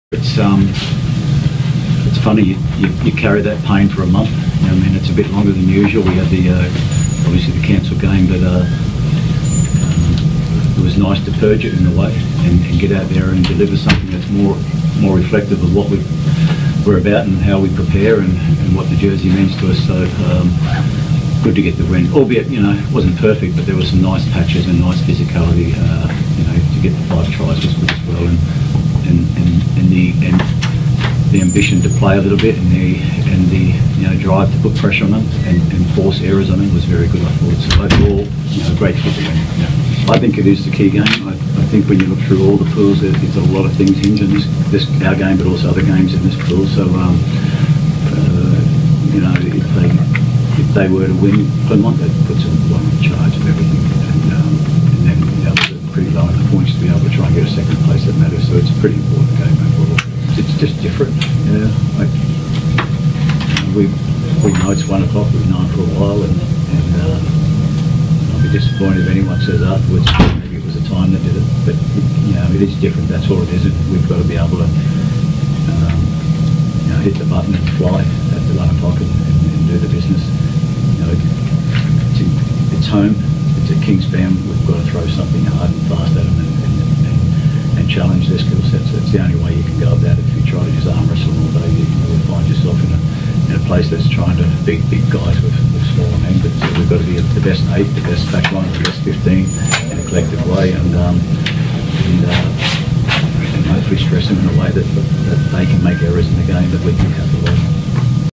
Ulster Rugby have a 'key' game on Saturday against ASM Clermont, says Les Kiss
By Kirsten Elder

Ahead of what he has described as the key game of his side's European season, Ulster Director of Rugby Les Kiss has admitted that preparing for a star-studded Clermont side has been a "reality check".
"You look at them and you can't help but be impressed by what they can offer," Kiss said.
"You admire them but it puts the fear of God in you, really.
"I'm not telling anyone anything new here really but we've had a reality check when we looked at the video this week."